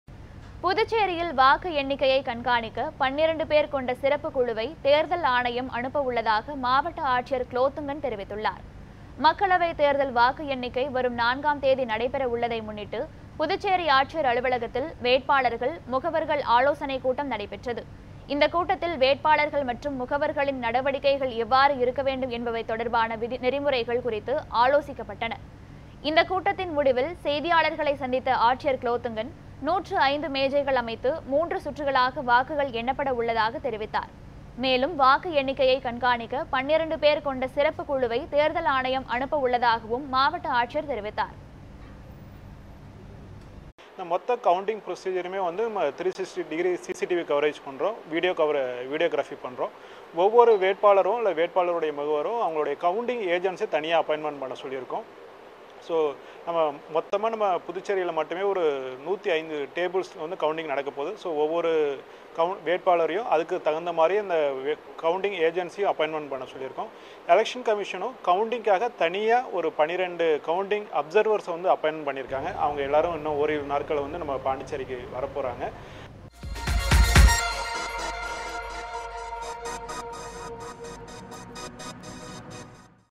"வாக்கு எண்ணிக்கை முடிவுகள் உடனுக்குடன் அறிவிக்கப்படும்" - குலோத்துங்கன், மாவட்ட ஆட்சியர்
புதுச்சேரியில் வாக்கு எண்ணிக்கையை கண்காணிக்க 12 பேர் கொண்ட சிறப்பு குழுவை தேர்தல் ஆணையம் அனுப்ப உள்ளதாக மாவட்ட ஆட்சியர் குலோத்துங்கன் தெரிவித்துள்ளார்...